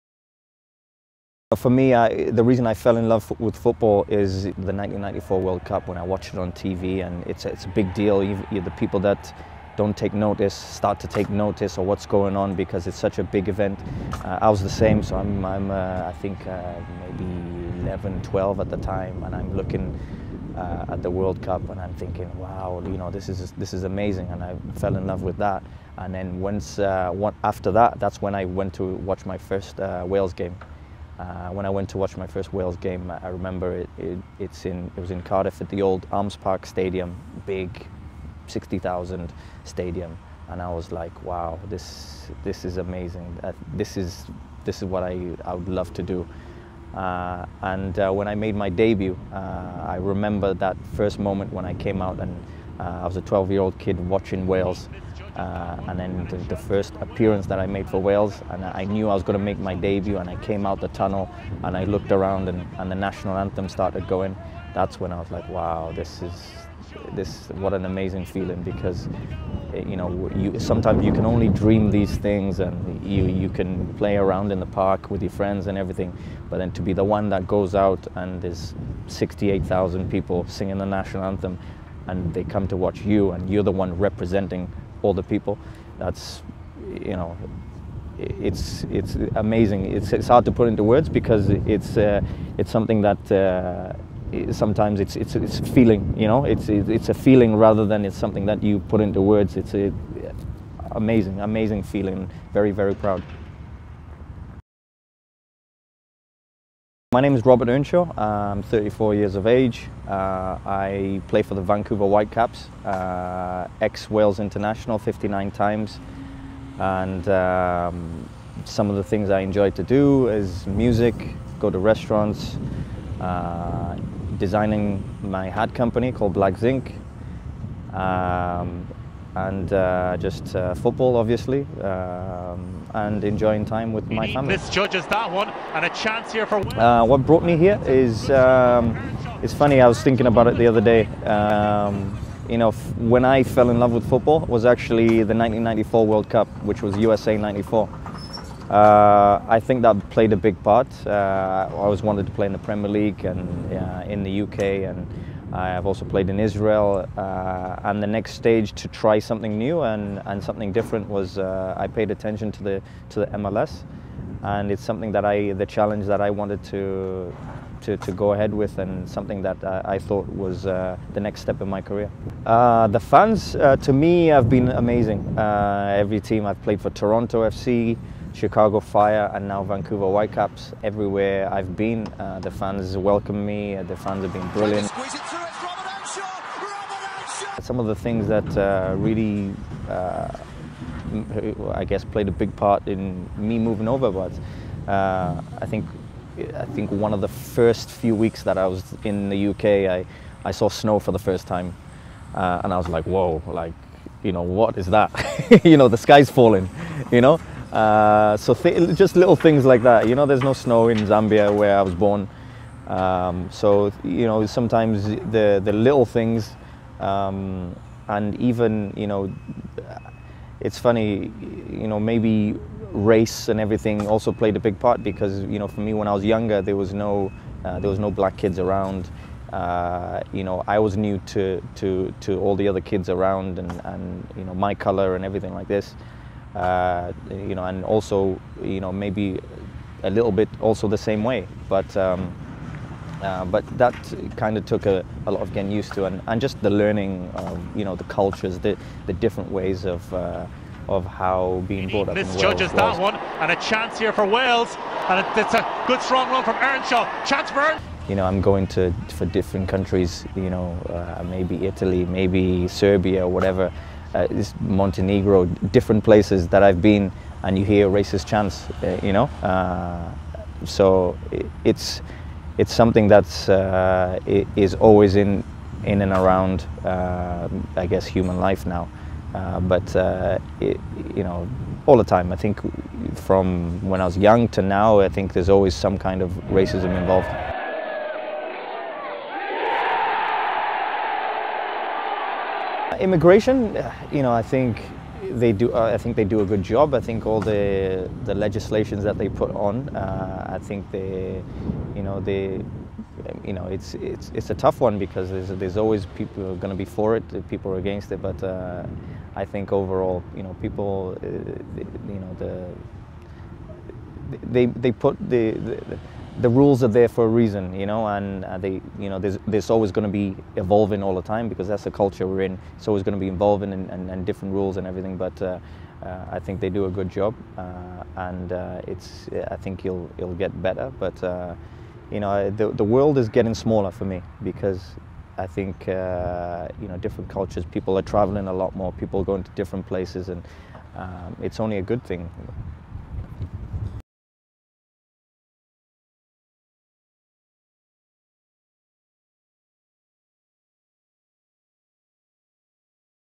Robert Earnshaw
This is the full interview with former Wales and Cardiff City player Robert Earnshaw who features in our WalesIsMyHome educational film.

Wales Is My Home is a short film, produced for Show Racism the Red Card by Helen Caplin (Break Every Chain Productions), exploring the controversial issues of immigration and racism. The film compiles personal accounts by individuals into the challenges faced by the UK’s migrant population today.